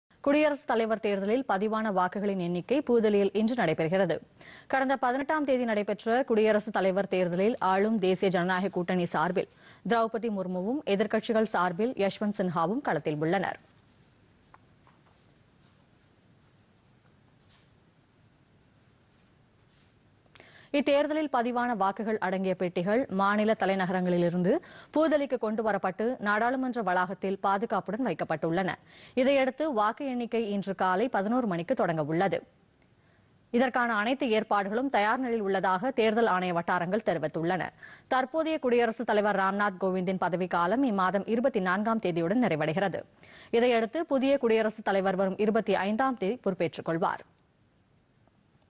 குடியரசுத்தலைவர் தேர்தல் - வாக்கு எண்ணிக்கை
நாட்டின் 16-வது குடியரசுத் தலைவர் தேர்தல் -  புதுதில்லியில் இன்று வாக்கு எண்ணிக்கை